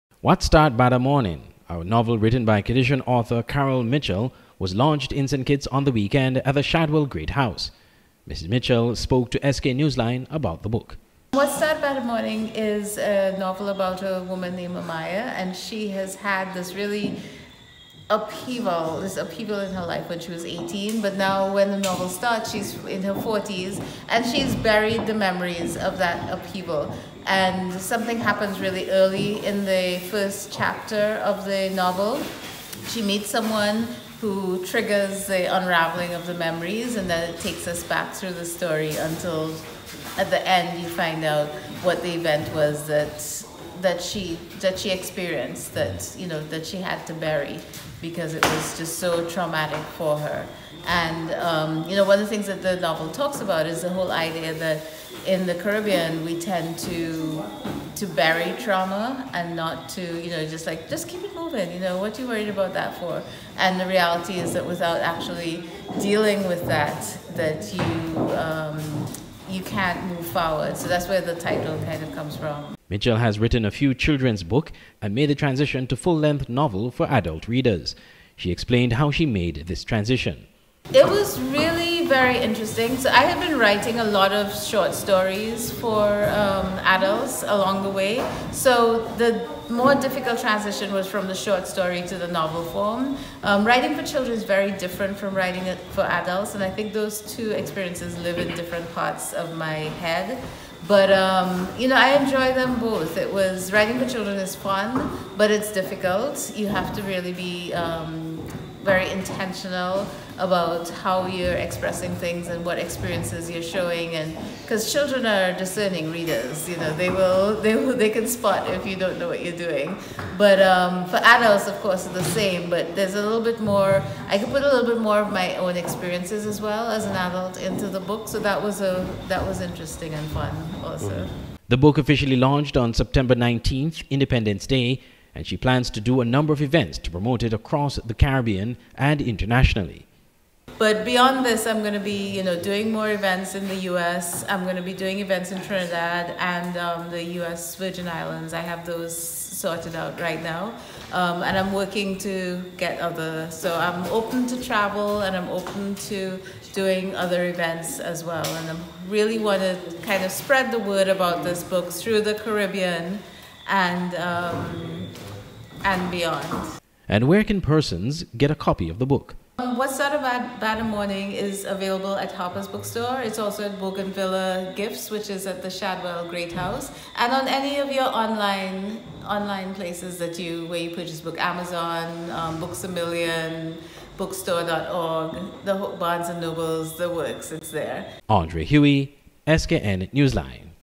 KITTITIAN WRITER LAUNCHES: ‘WHAT START BAD A MORNIN’
What Start Bad a Mornin’, a novel written by Kittitian author Carol Mitchell, was launched in St. Kitts on the weekend at the Shadwell Great house. Mrs. Mitchell spoke to SKN Newsline about the book.